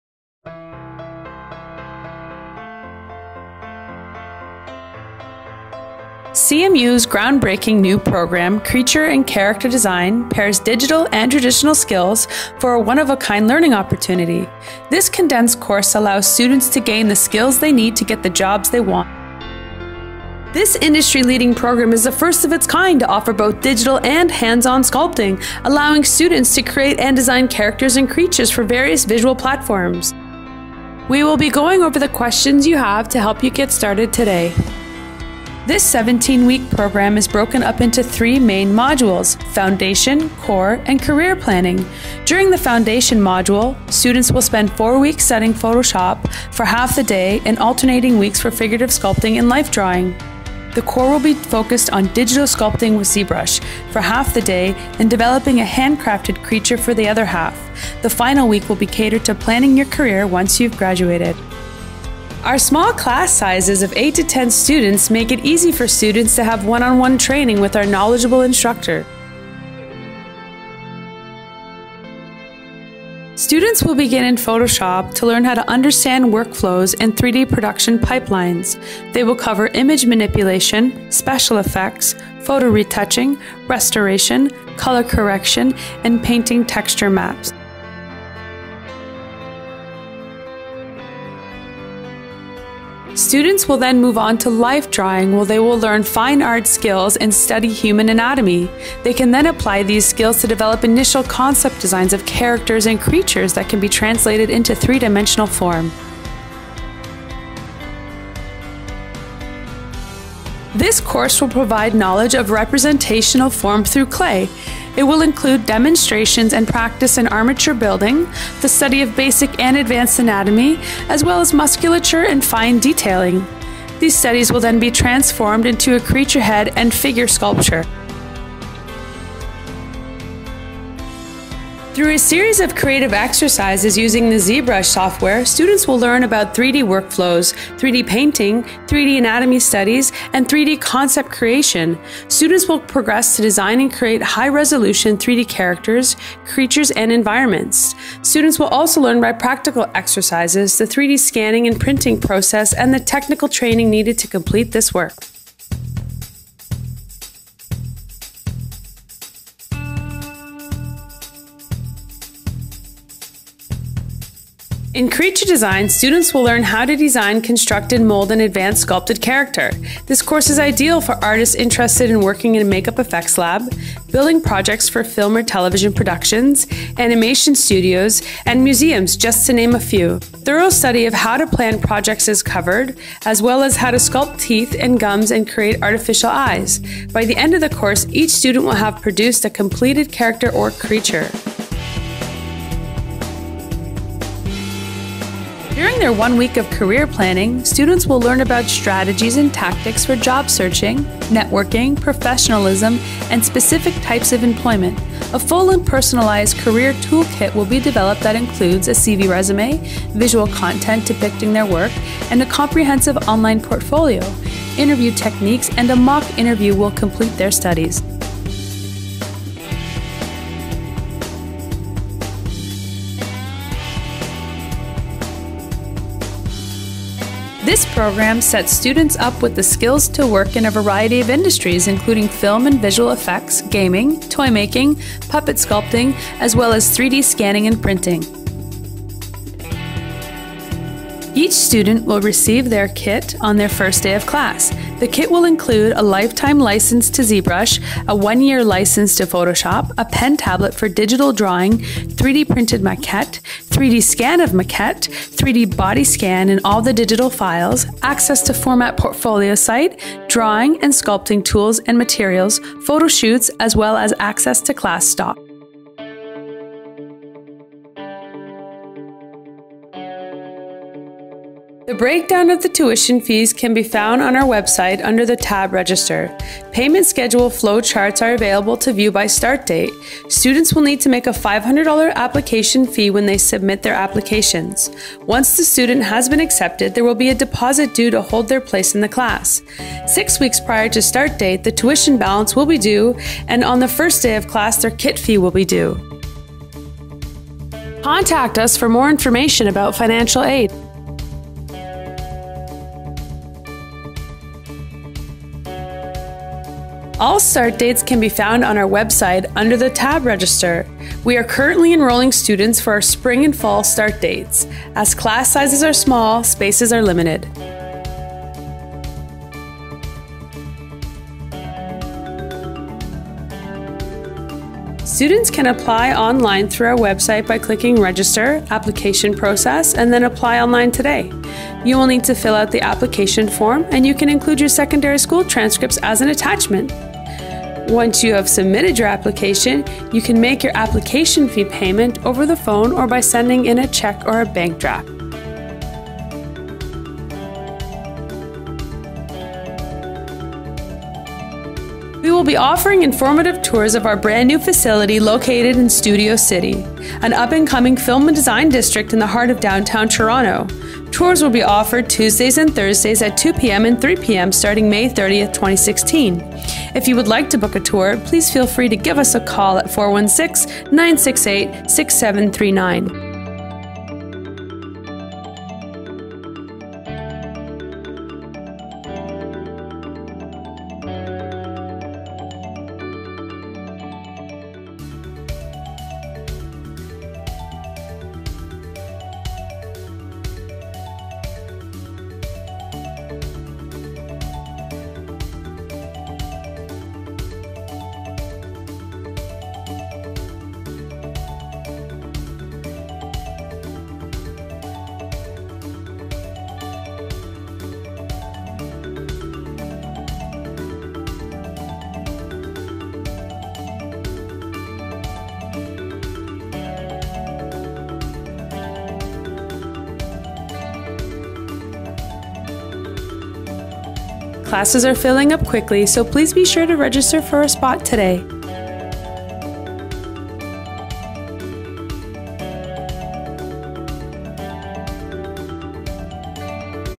An Inside Look At: Creature and Character Design
CMU's NEW Creature and Character Design Program combines the study of hand-crafted creature design with digital sculpting through ZBrush. This unique program provides students with a unique skill set to work in industries like Film, Animation, Gaming and 3D Printing.